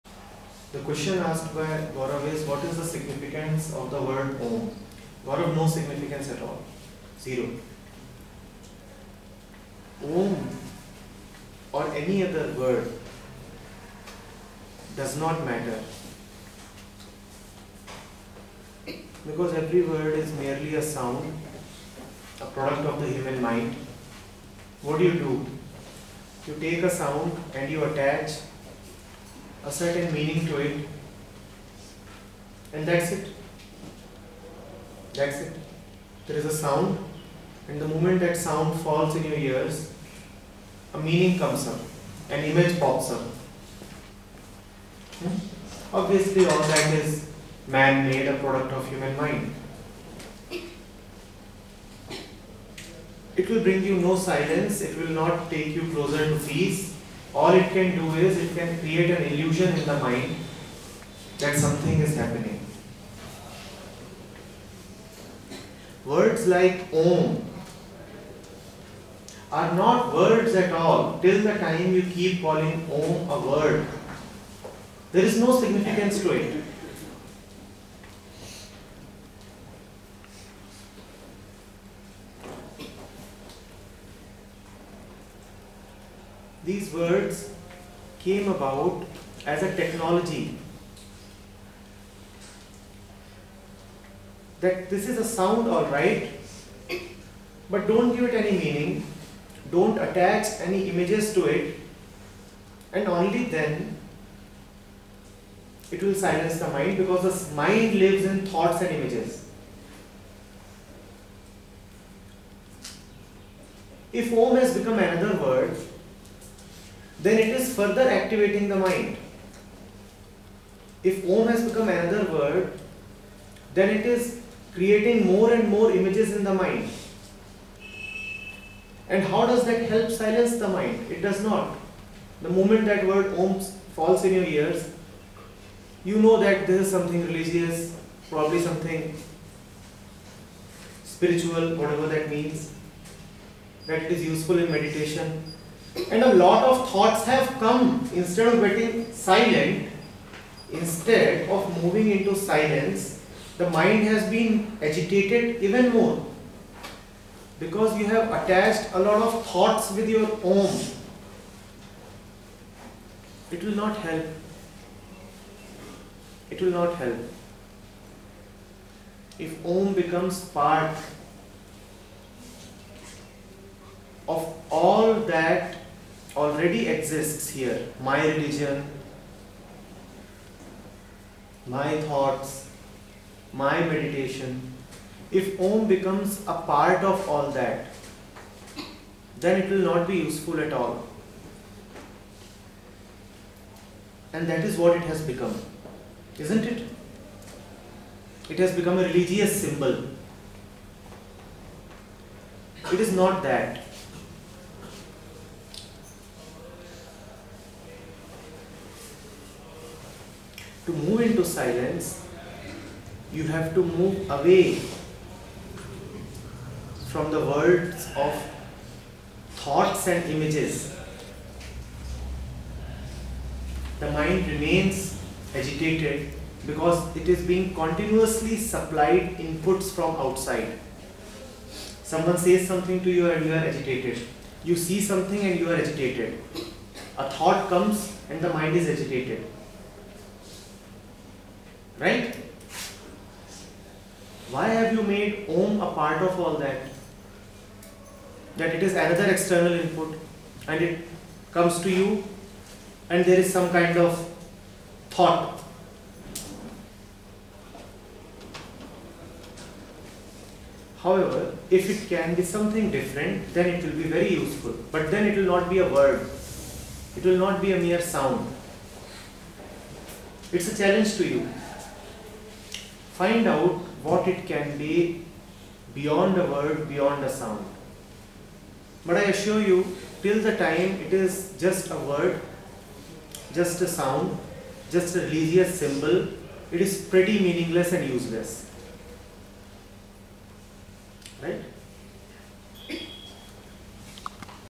What is the significance of words like 'Om'? || Acharya prashant, with youth (2013)
Video Information: Samvaad Session, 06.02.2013, Moradabad, Uttar Pradesh, India

Context:

What is  the meaning of 'Om'?
What is the significance of words like 'Om'?
Is 'Om' a religious word? .